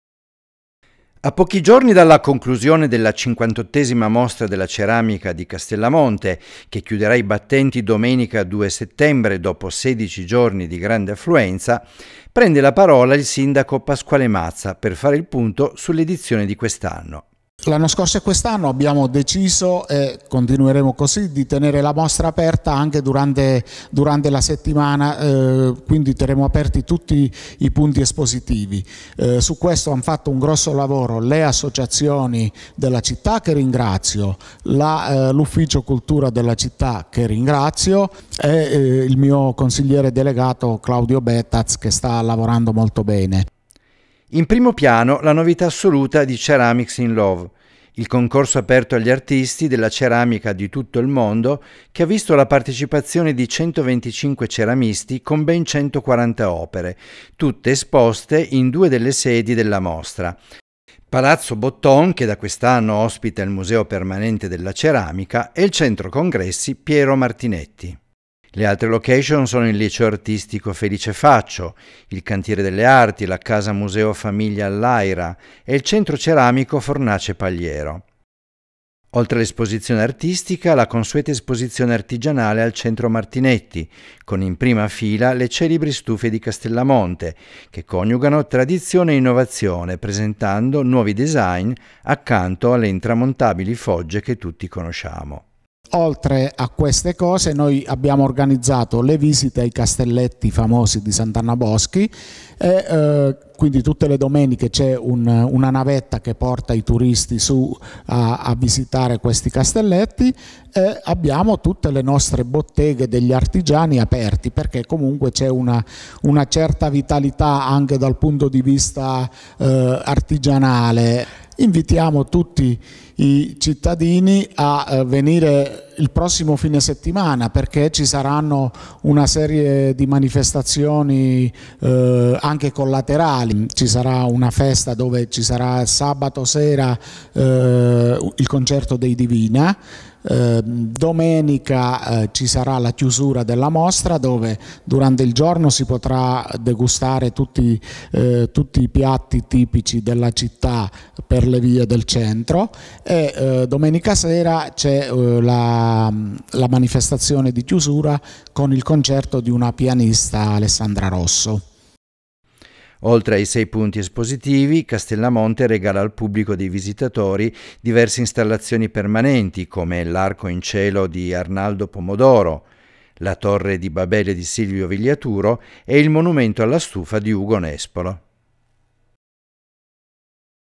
58° mostra della ceramica di Castellamonte
Aperta fino al 2 settembre l'importante manifestazione che raccoglie opere d'arte da tutto il mondo e manufatti dell'artigianato locale, come le celebri stufe di Castellamonte.